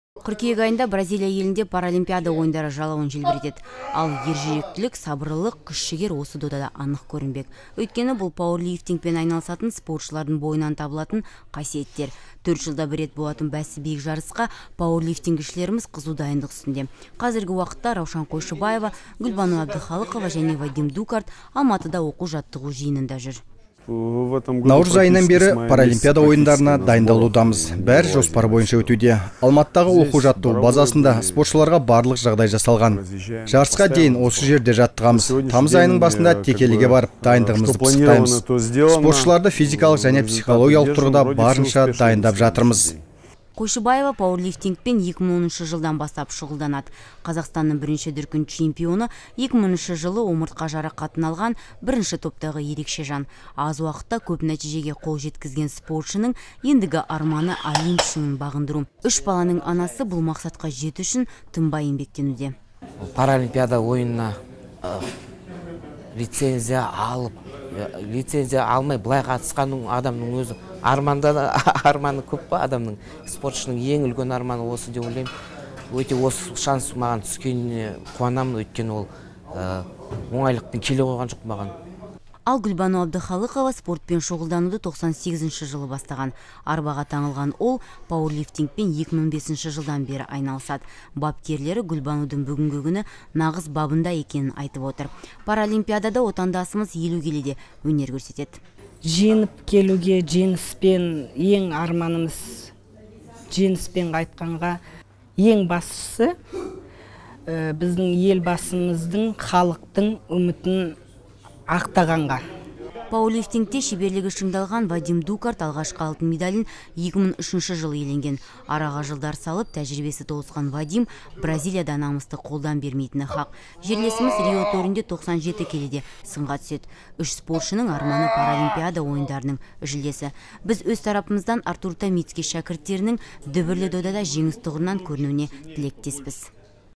Pauerlifting kuramasy 01 08 16
Немного о пауэрлифтинге и наших атлетах Паралимпийцах в серьезнотяжелом виде спорта: Пауэрлифтинг.
Абдыхалыкова Гульбану, Койшыбаева Раушан и Дукарт Вадим-НАСТОЯЩИЕ "ЛЮДИ ИЗ СТАЛИ".
Именно им выпала честь представлять нашу страну на Паралимпийских Играх в Рио2016.
‪#‎Rio2016‬ ‪#‎kazparalympics‬ Paralympic Games ‪#‎NPC‬ ‪#‎Astana‬
National Paralympic Committee of Kazakhstan - "вся жизнь в движении" "life is a movement"